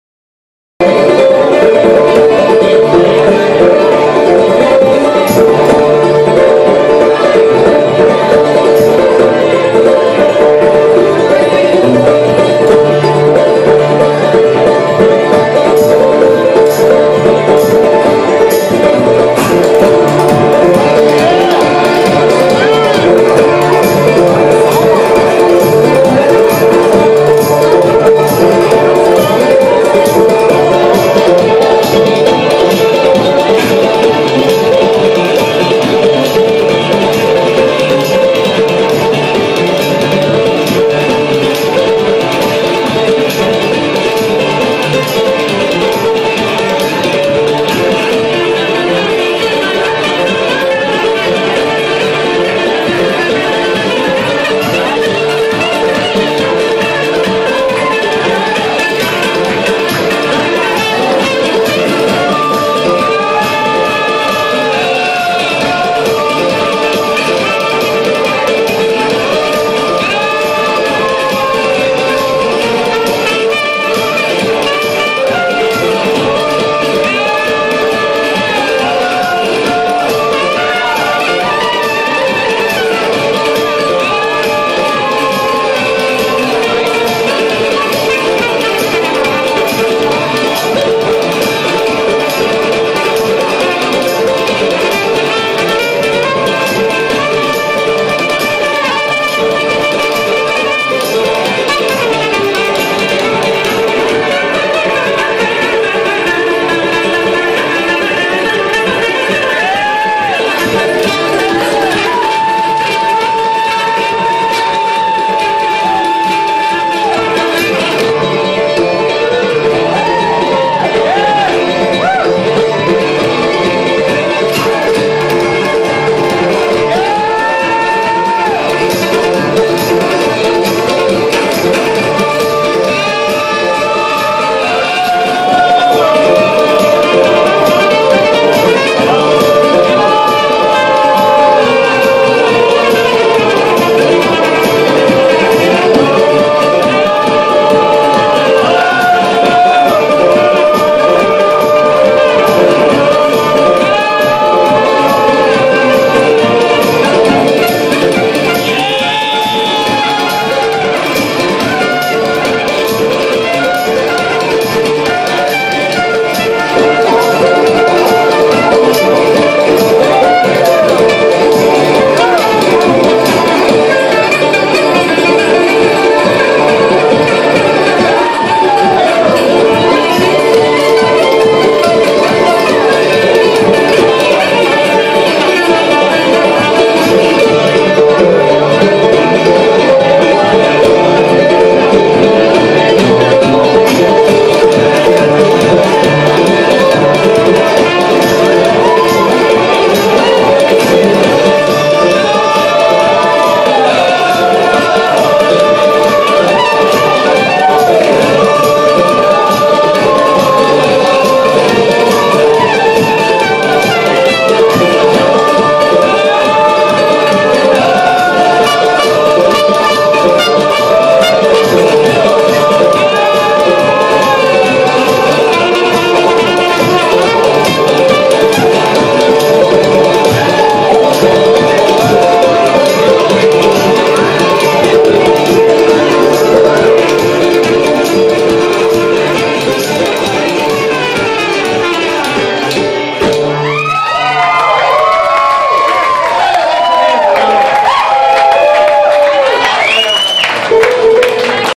Whispering Beard 2013 - Morgan O' Kane
Thursday night in the barn. Getting warmed up for 3 days of music and fun. Morgan O' Kane and his band tearing it up in Friendship,  Indiana!